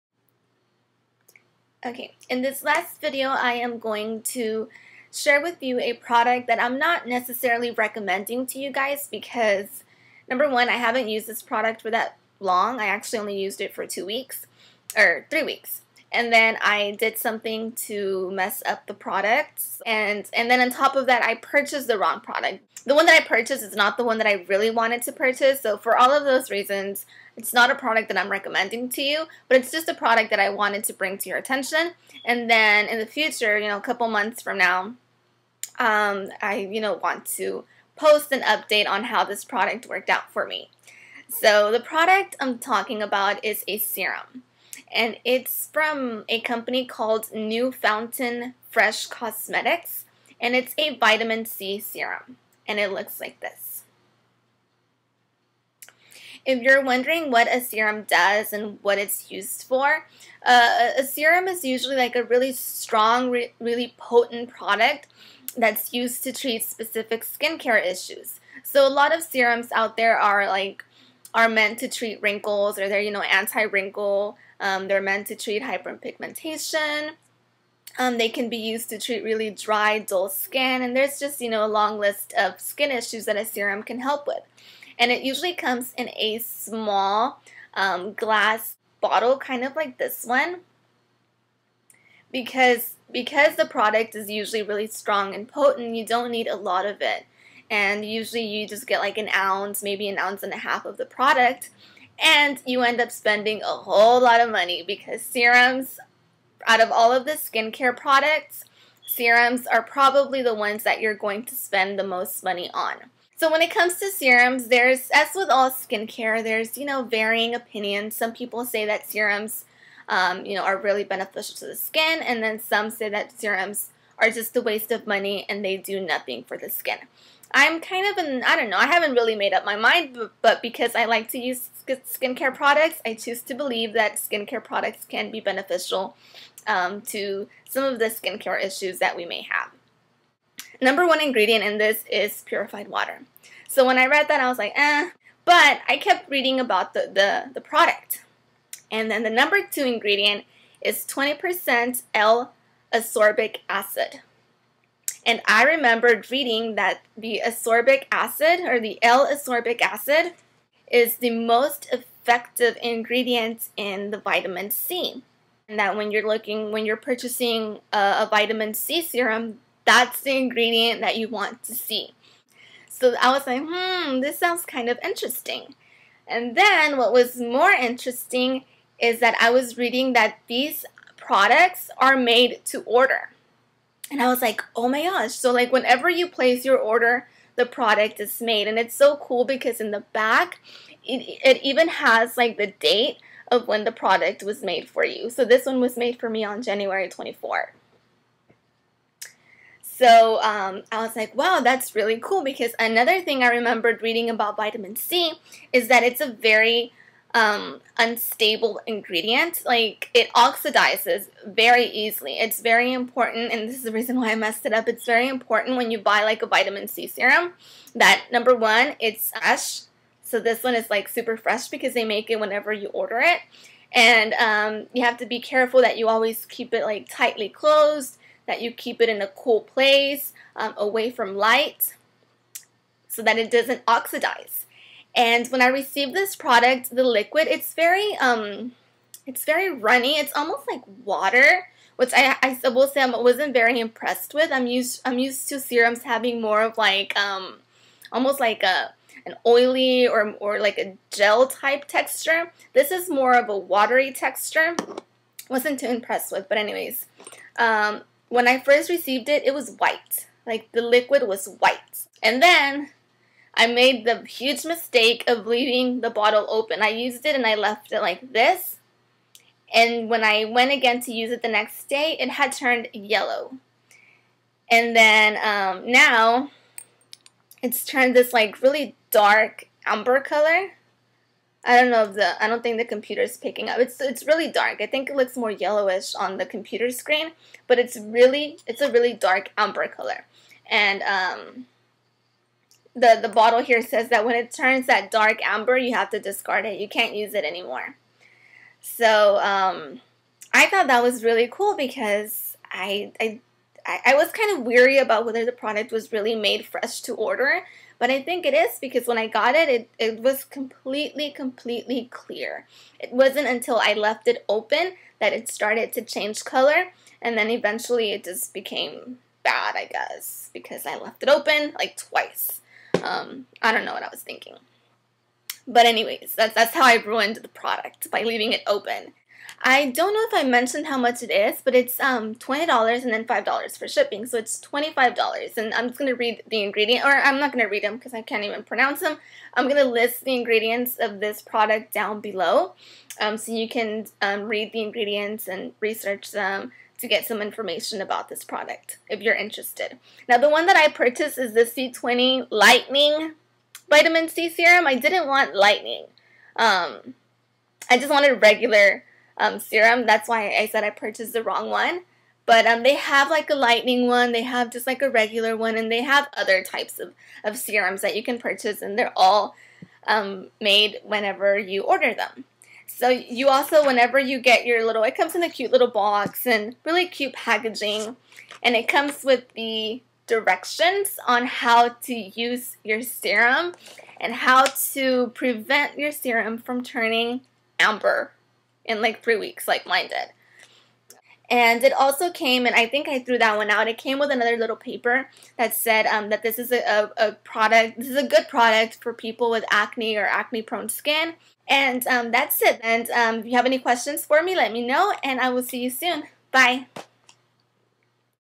Skin Care Products: Vitamin C Serum (Part 4)
NuFountain Fresh Cosmetics C20+ Lightening Vitamin C Serum

Purified Water, 20% L-Ascorbic Acid, 6% Alpha Arbutin, 6% Niacinamide (Vitamin B3), Citric Acid, Magnesium Sulfate, Glycerin. pH of 3.25